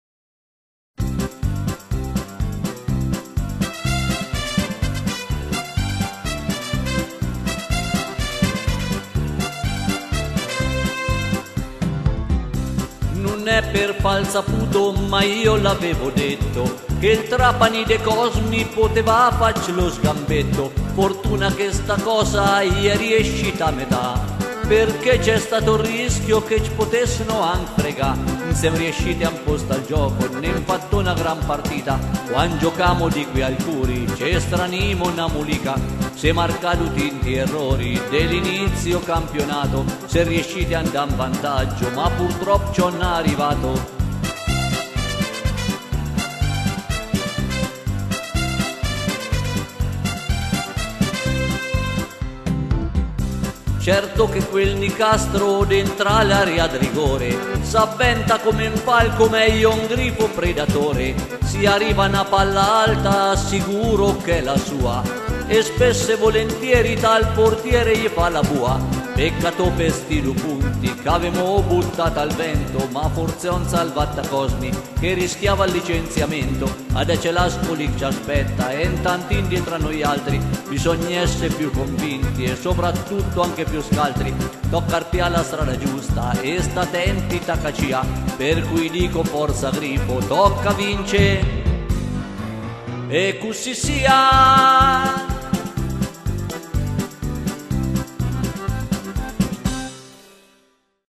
Rocosintesi - Perugia Trapani 13.11.2016 con testo
Sintesi Musicale della partita Perugia Trapani del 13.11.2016